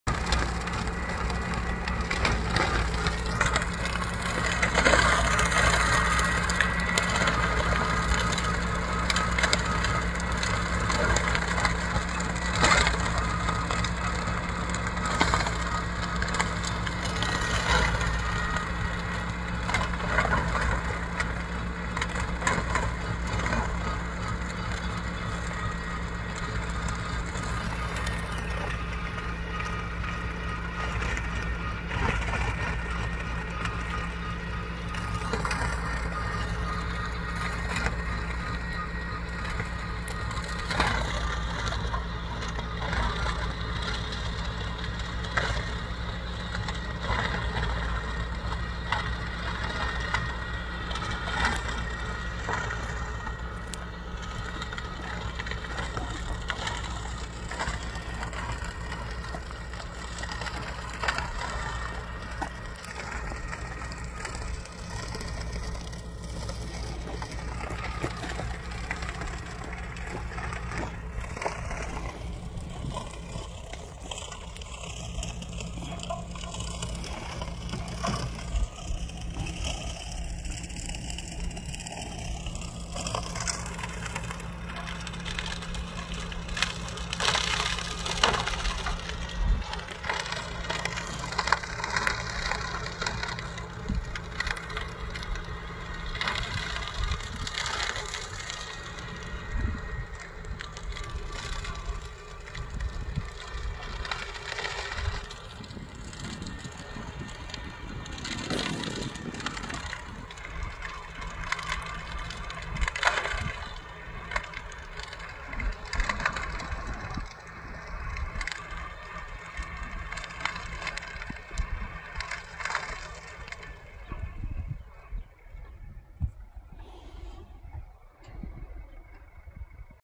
TUTKUN Manure Spreader
TUTKUN Manure Spreader loads manure, and provides a uniform way of spreading it on the fields.
When the machine is loaded, the specially designed suspension system on the chassis, prevents the tractor from the oscillations that will occur, and in this way it provides a more comfortable driving experience.
Specially designed vertically positioned beaters, with the blade attached to them, provide a homogeneous spreading of the muck on the field.
Also, the specially designed transmission system that allows the transfer, is designed to work without any problems and can also withstand high power capacity for many years.